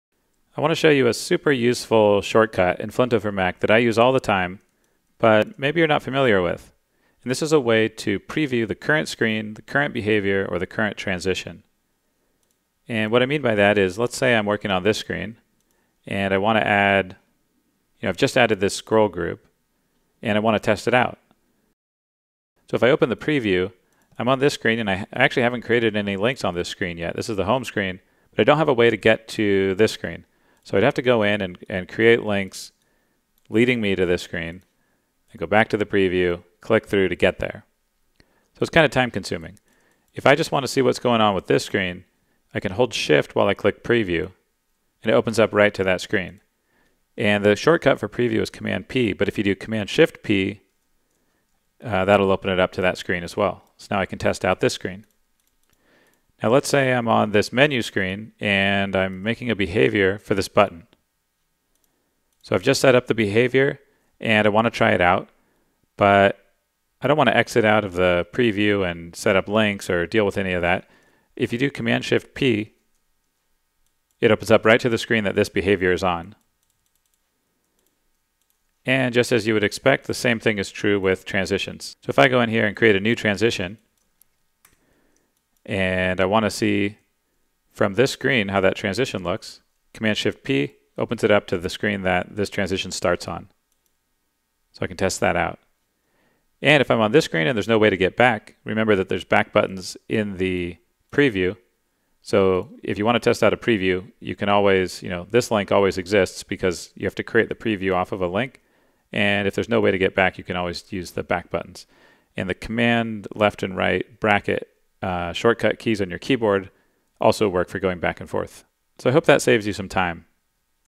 Preview selected screen - time saving shortcut
Use the shift key to open a preview to the current screen, behavior or transition.